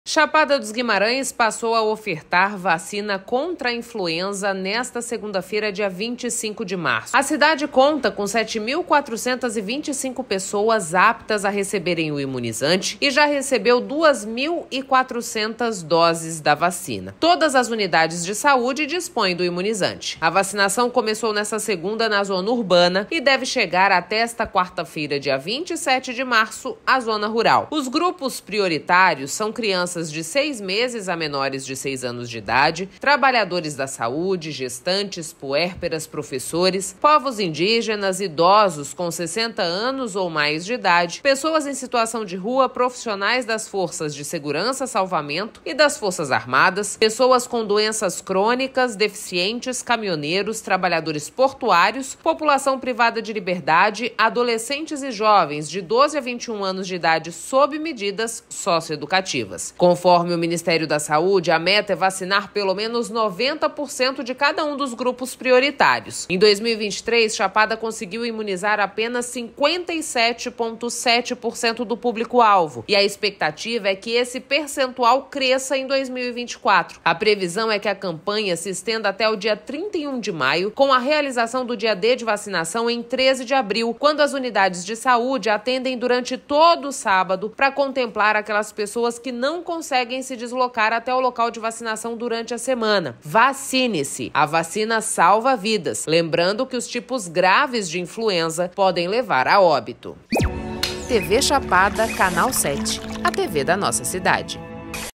Vacina da gripe já está disponível para grupos de risco em Chapada
Chapada dos Guimarães passou a ofertar vacina contra a Influenza nesta segunda-feira (25). A cidade conta com 7.425 pessoas aptas a receberem o imunizante e já recebeu 2.400 doses. Todas as unidades de saúde dispõem do imunizante. Confira:

Integre o grupo de WhatsApp da TV Chapada.